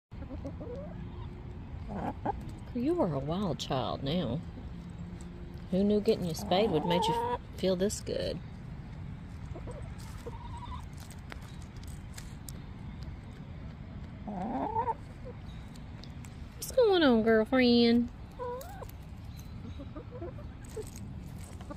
Autumn has never gotten this close while I was sitting down. Progress! 😲
Since Autumn came home from her surgery, she's been so much more affectionate. I've been able to touch her tail a few times, and she rubs against my legs every time I walk outside. I hope one day soon she can come inside!